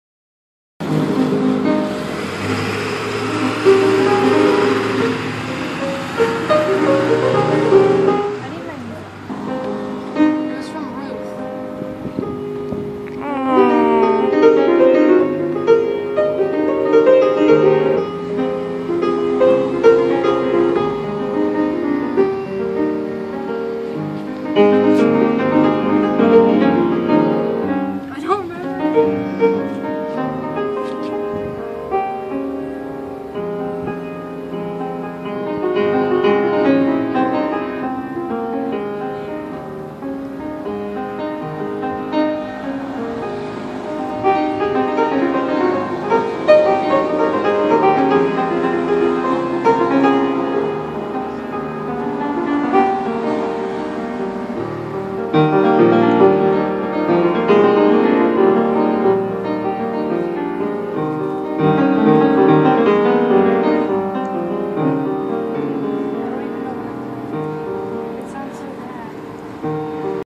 Jacqueline playing the painted pianos downtown.
Sorry for the poor camera job. My fail.